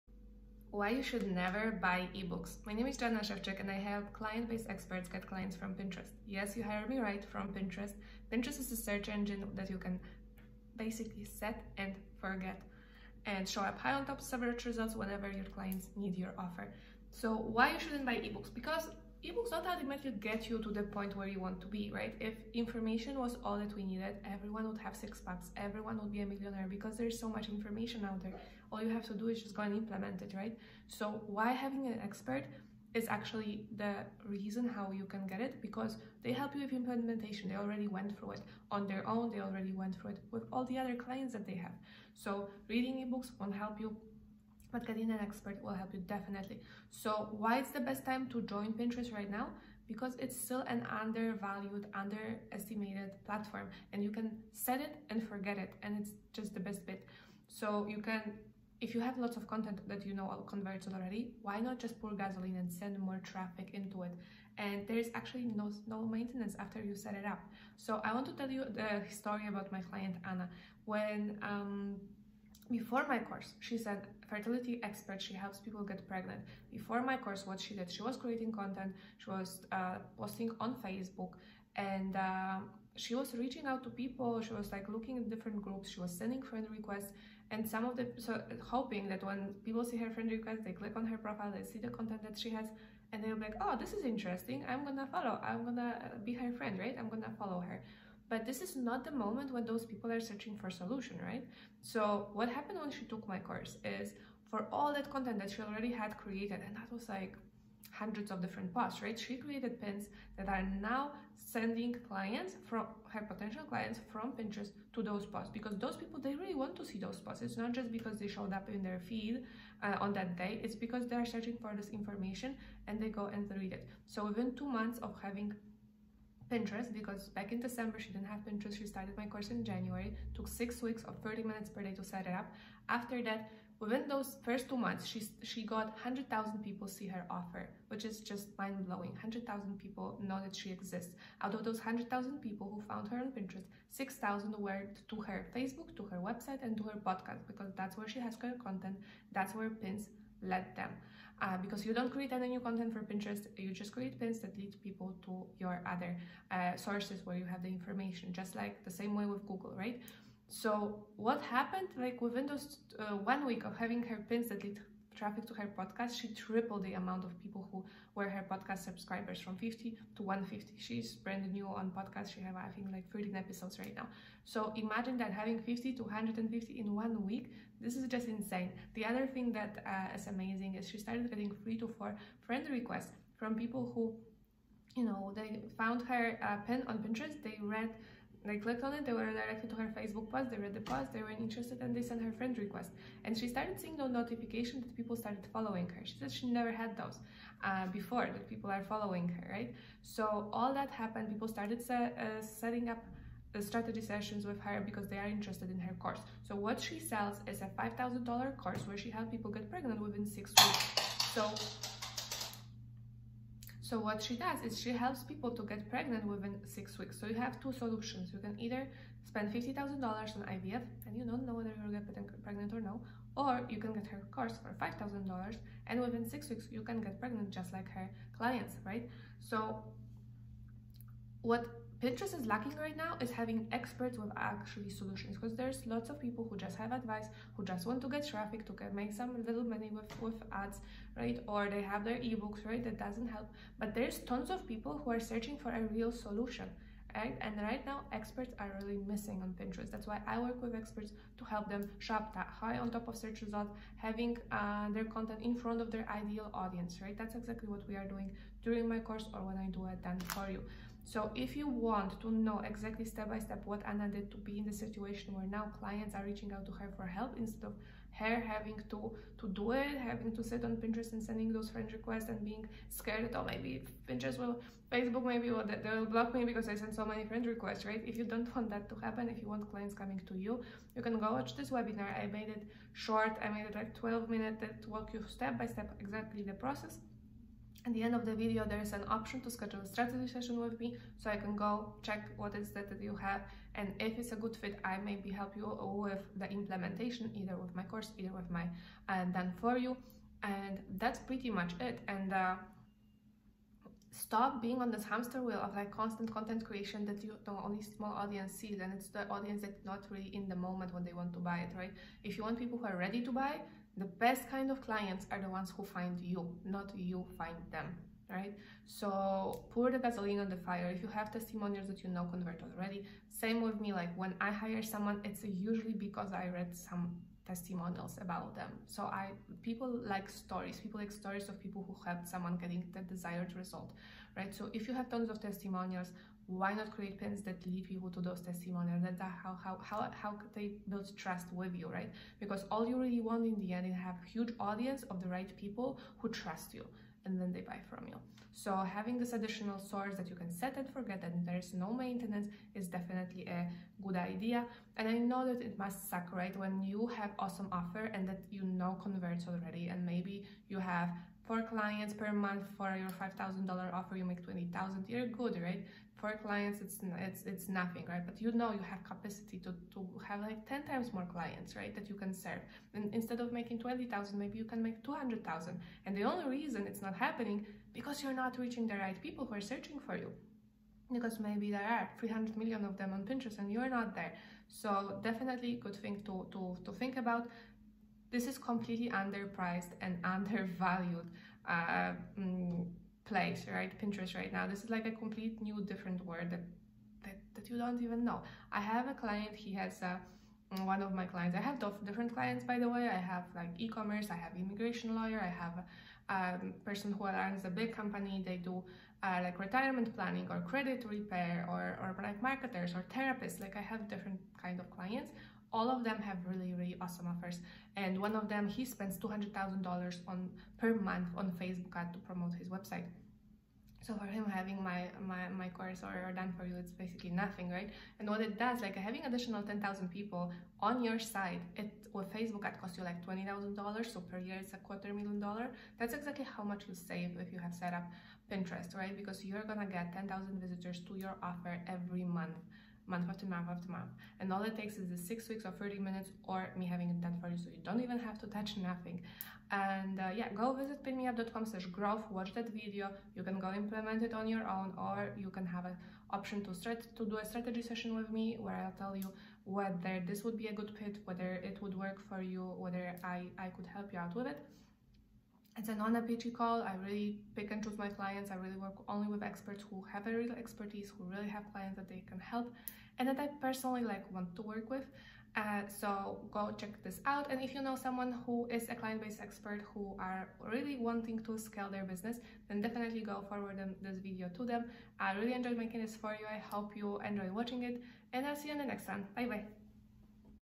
Why you should never buy ebooks | PinMeApp Pinterest Tips
🗣 Interact with me on a personal level!

🚨About:  In this video, I share with you how come you put in a lot of time and effort and are still stuck/in the same place?

1️⃣The secret system which allows me to get 10,000+ free targeted visitors to my website from Pinterest every month (and how you can steal the system for yourself)
7️⃣How much money are you loosing by not having a Pinterest account set up